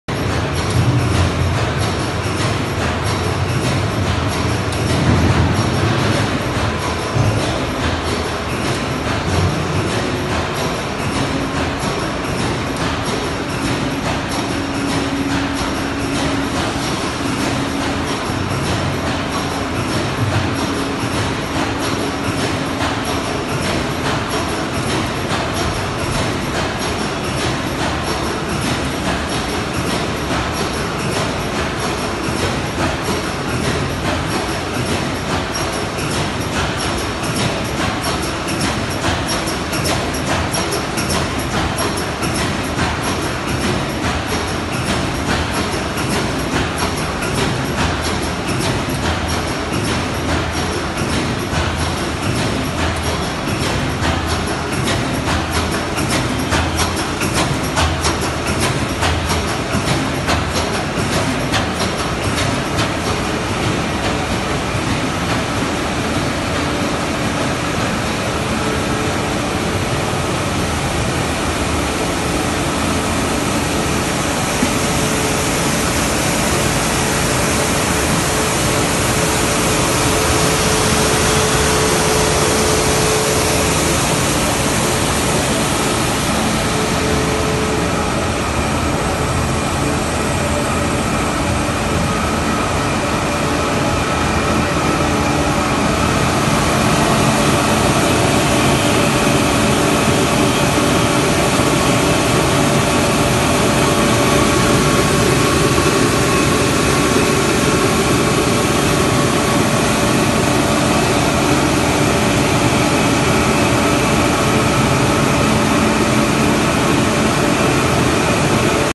COEX vormmachine
Verbufa COEX forming machine for stuffed meatballs

Verbufa COEX-vormmachine voor gevulde vleesballetjes